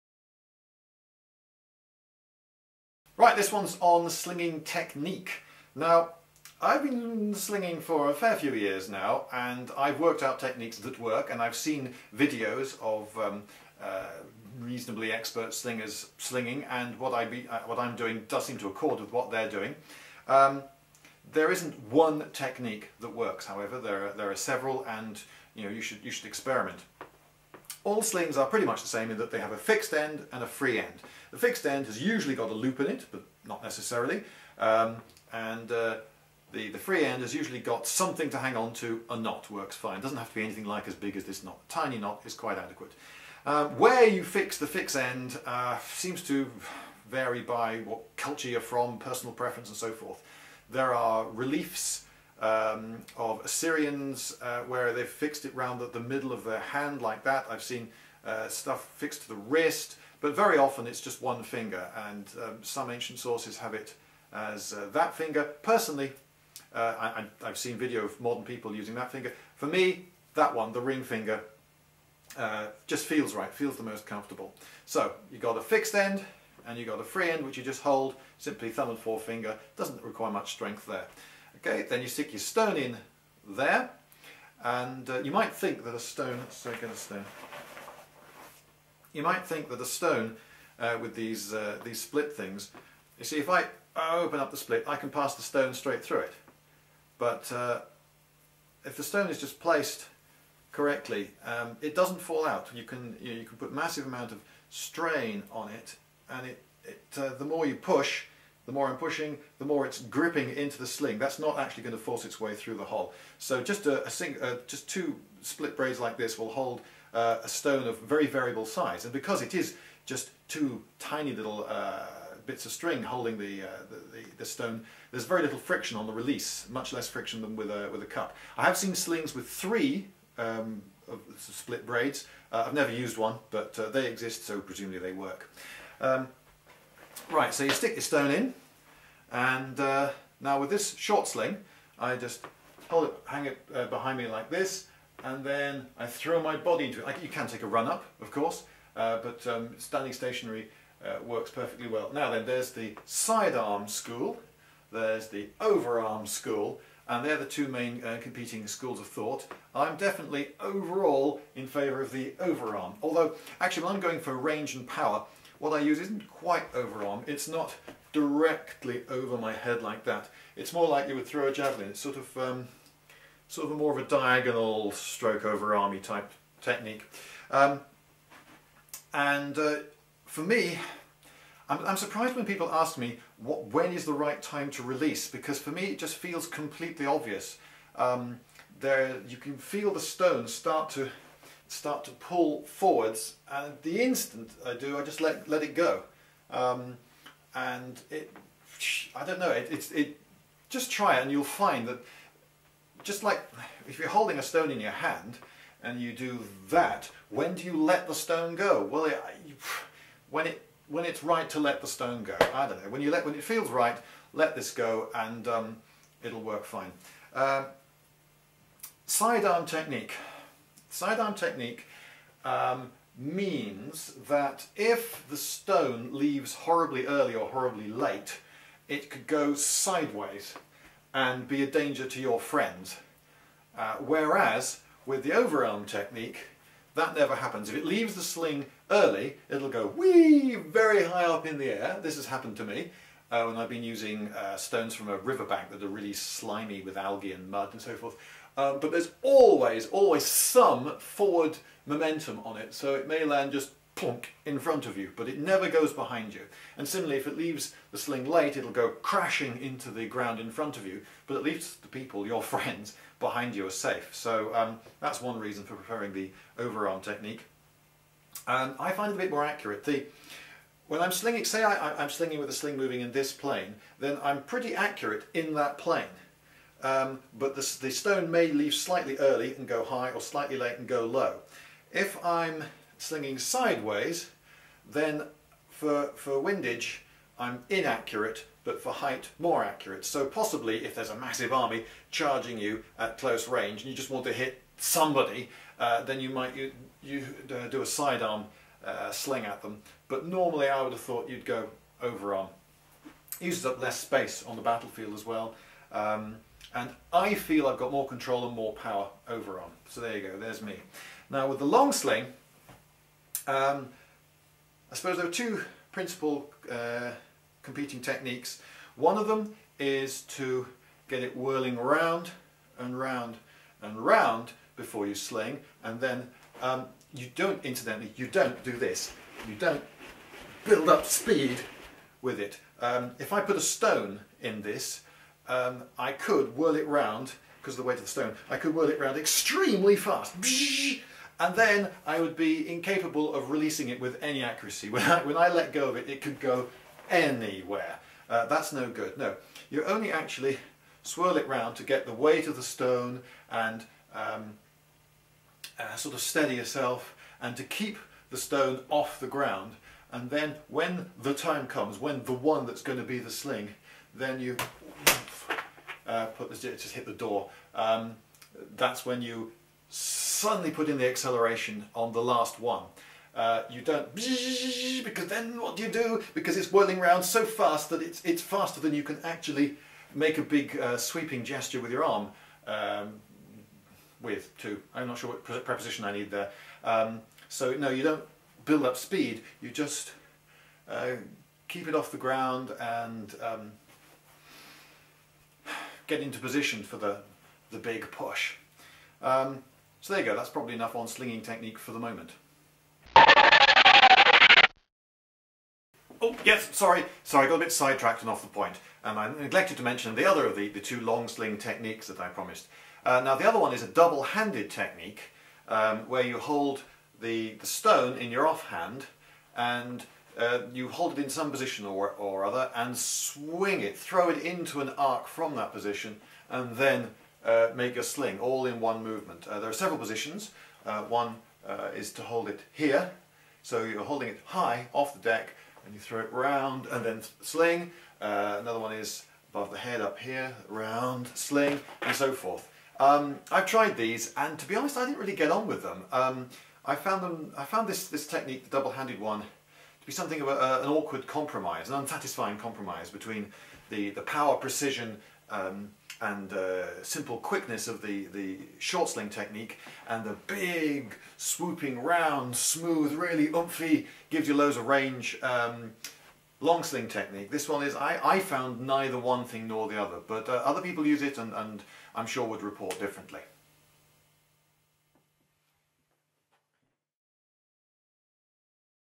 Slinging techniques
Another of my series on slings - this time the techniques of using them.

This one turned out a bit longer than I had intended.  Sorry about that.  I try to keep them down to five minutes or under, but this one got away from me.  At one point I start to repeat myself, and then grind to a slow halt, which I think is symptomatic of a man who is sure he meant to say something more, but can't think what it was.  I remembered some while later, and had to get back into costume to shoot the extra bit.

I'm doing rather a lot on slings, because I think that knowledge of them is comparatively rare, and I have a lot I could say about them.  At least two more to come.

My website has small video files of my using a sling on Newcastle Town Moor